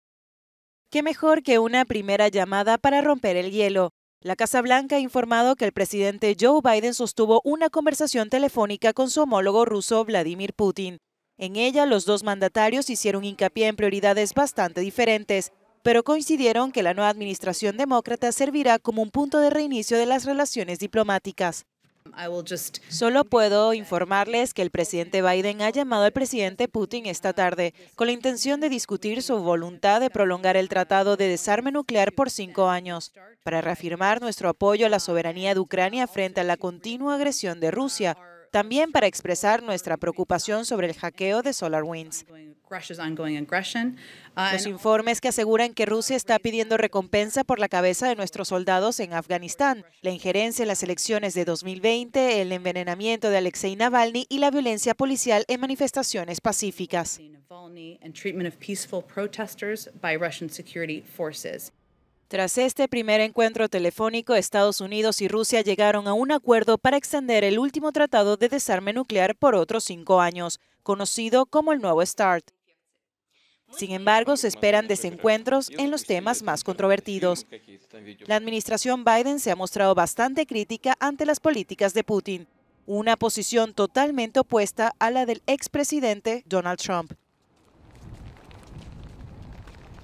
Biden y Putin acuerdan extender el 'Nuevo Start' tras primera conversación telefónica
Los dos mantarios hicieron hincapié en prioridades bastante diferentes pero coincidieron que la nueva administración demócrata servirá como un punto de reinicio de las relaciones diplomáticas entre Estados Unidos y Rusia.